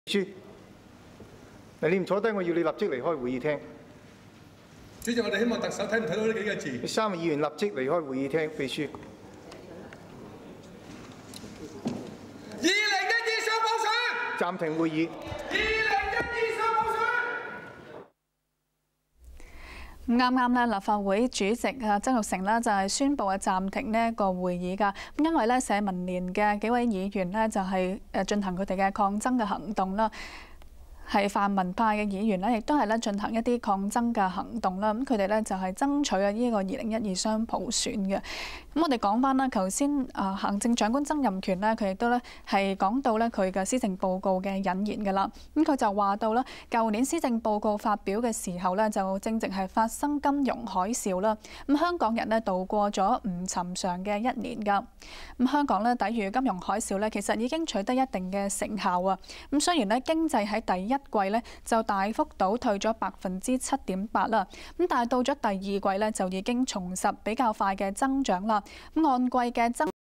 抢劫穷人保富商之特首共产党尻政报告2010 十月十四日 早上十一時
毓民: "群賊創新天!窮人墮深淵!"@2010無料到自私政報告-特首答問會15OCT09